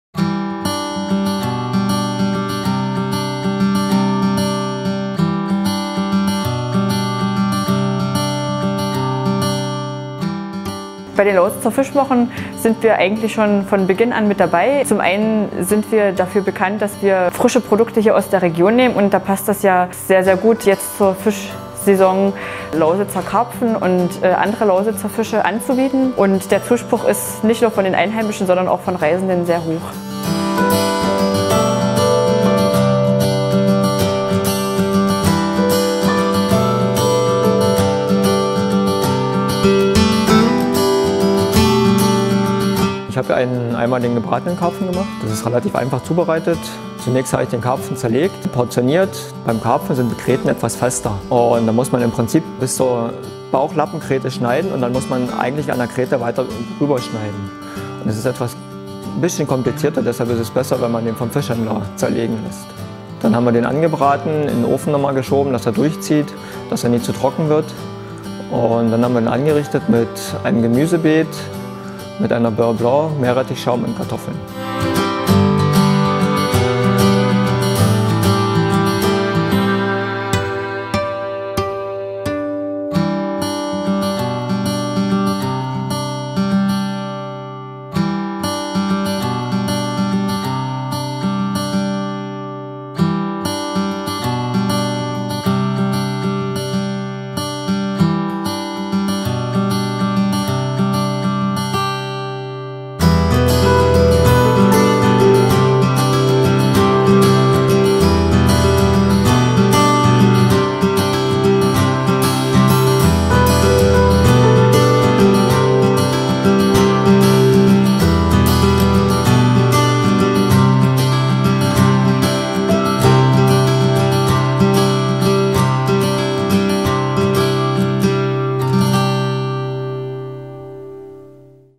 Lausitzer Fischwochen 3 von 4 - "... einmal den gebratenen Karpfen."
Besuch im sorbischen Restaurant "Wjelbik" in Bautzen während der Lausitzer Fischwochen 2018 + Lausitz + Karpfen + Oberlausitz + Oberlausitzer Heide- und Teichlandschaft + Lausitzer Seenland + Östliche Oberlausitz + Fisch